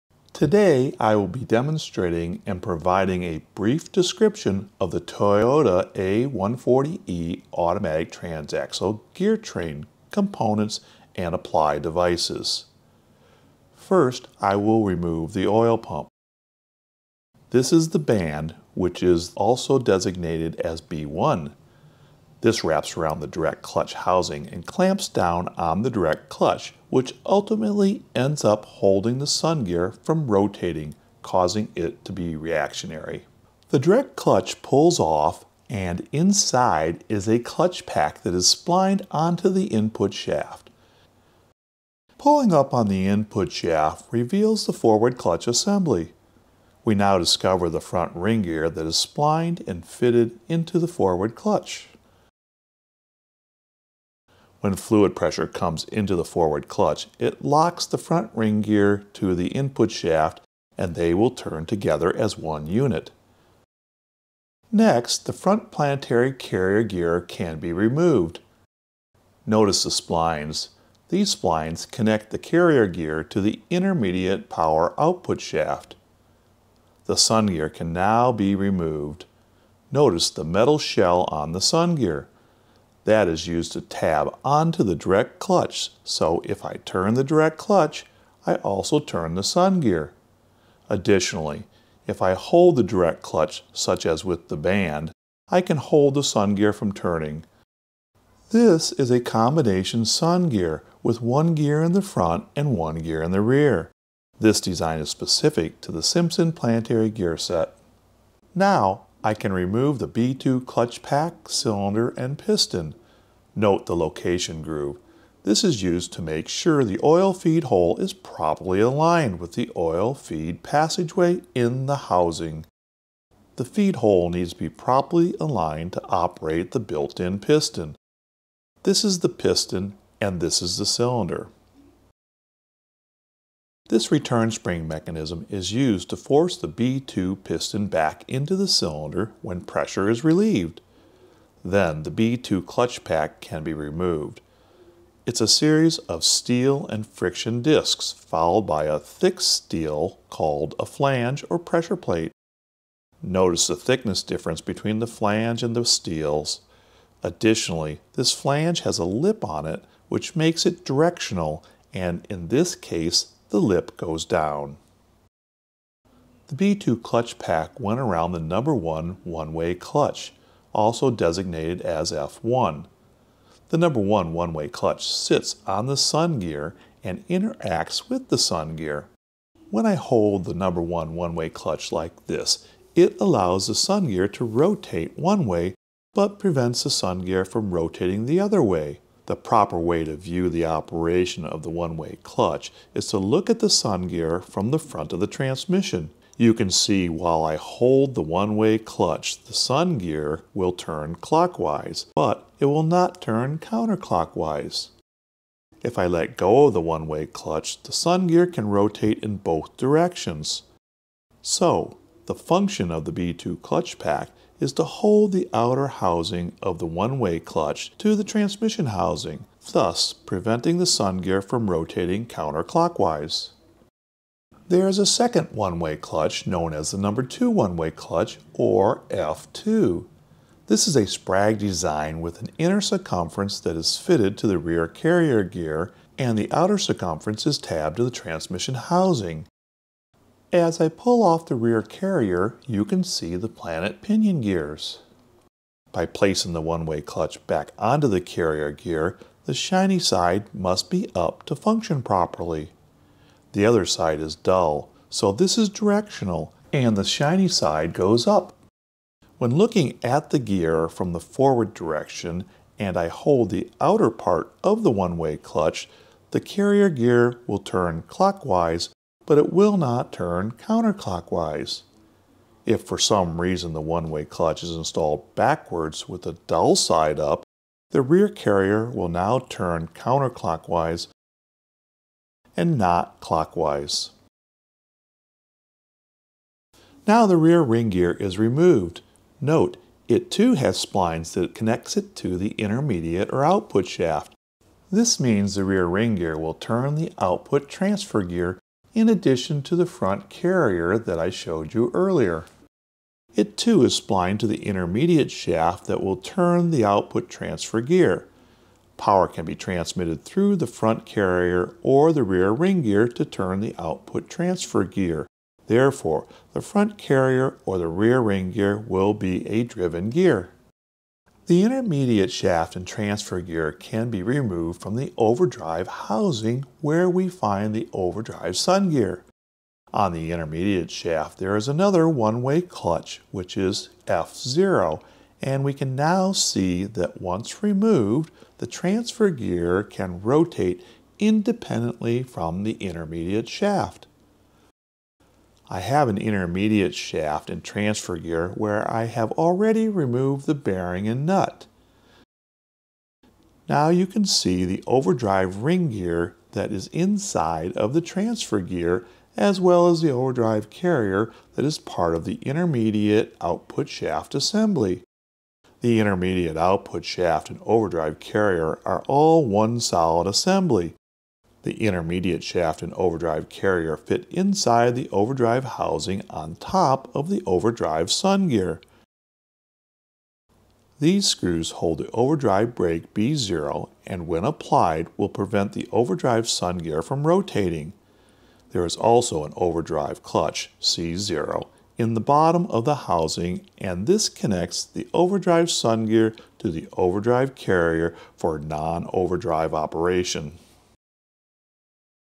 Toyota Automatic Transaxle Power flow Components Explained
2nd video is a playlist series that explains how the Toyota A140E Automatic Transaxle with the modified 4-speed Simpson power flow and gear train works with clutch packs and one-way clutches.